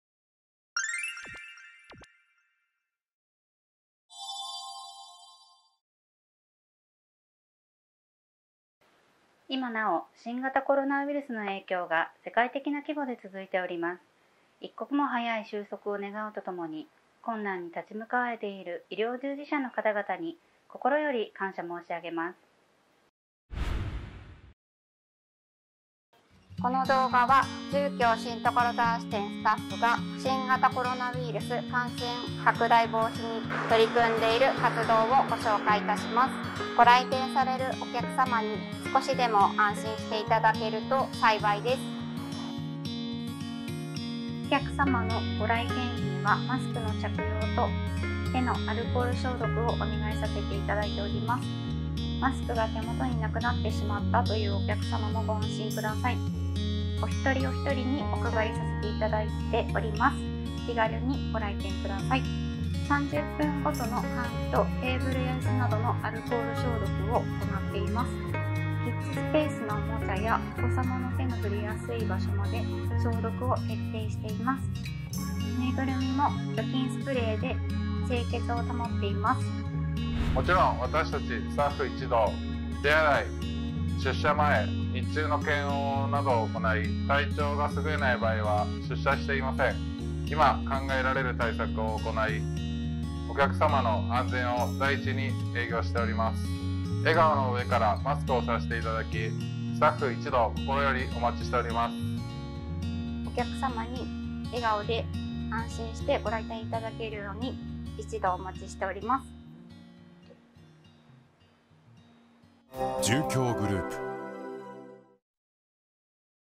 新型コロナウィルス感染症に関する「住協グループの取り組み」【住協】
埼玉・東京で不動産をお探しの方は住協グループの住まい情報をご覧下さい！

住協グループにおける新型コロナウイルス感染防止対策について

お客様に安心してご案内・ご来社をしていただくために、
株式会社住協 新所沢支店の取り組みについて動画でご紹介させていただきます。